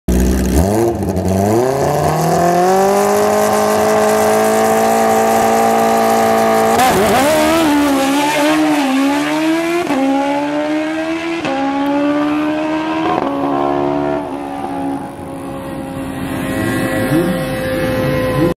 2015 IDBL - Friday Testing - Lee Shierts 8.60
IDBL - International Drag Bike League
29th Annual MTC Summer Nationals
Maryland International Raceway

Photo by Hot Rod Chrissy